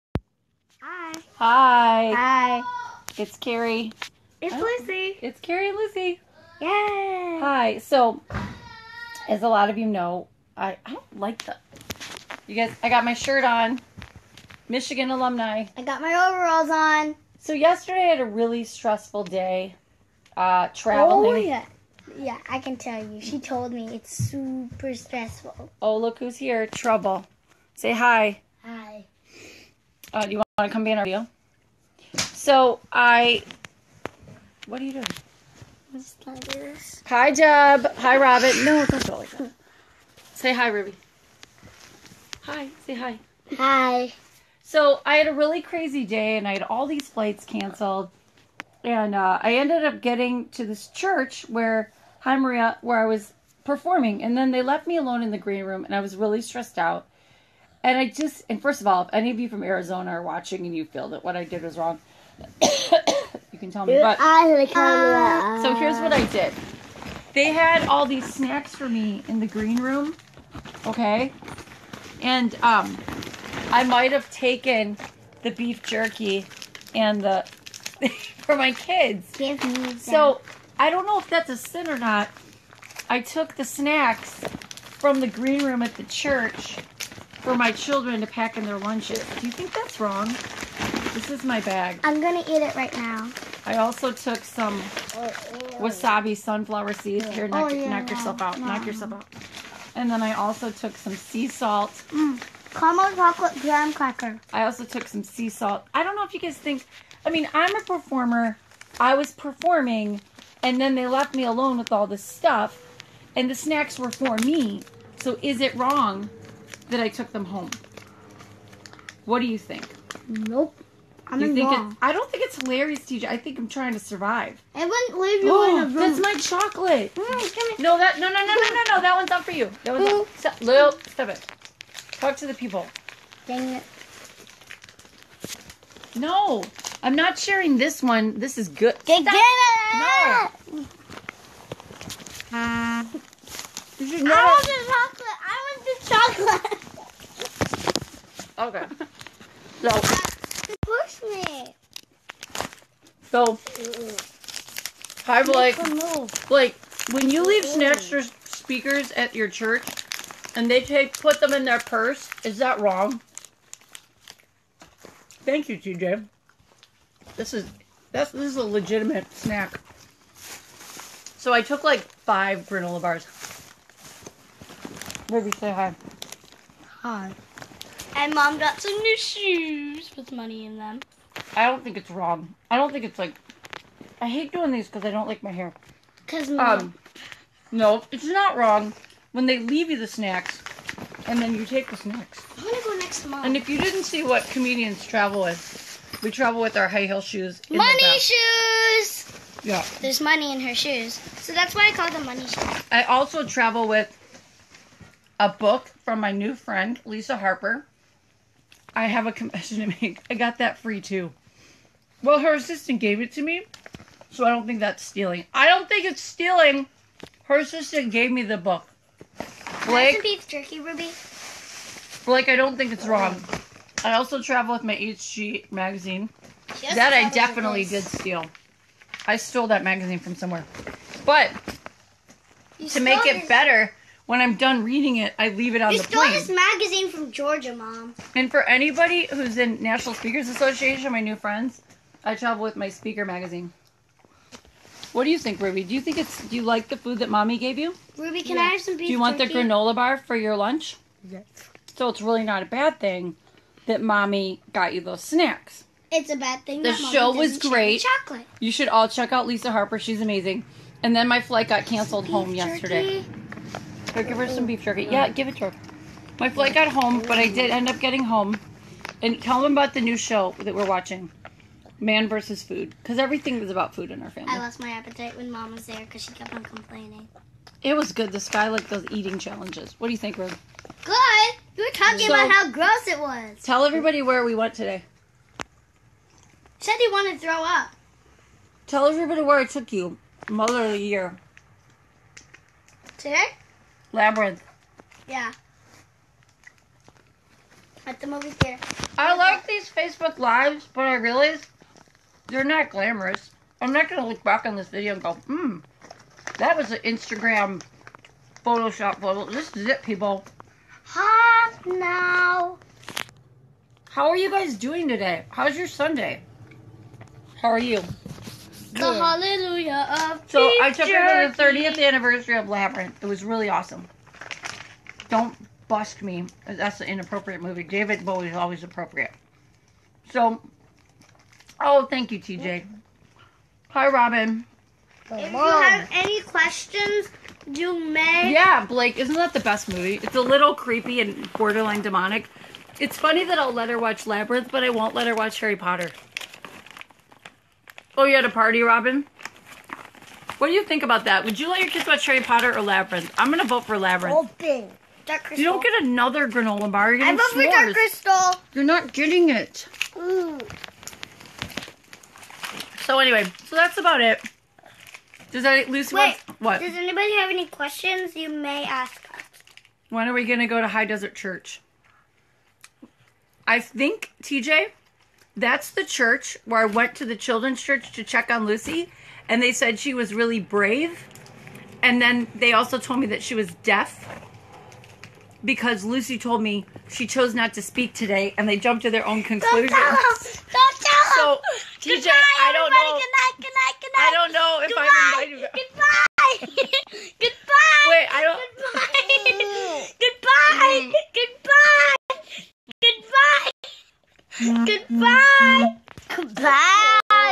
Stealing Things and Stress Eating
Sometimes you just gotta steal a few things and stress eat with your children. Is that really so bad?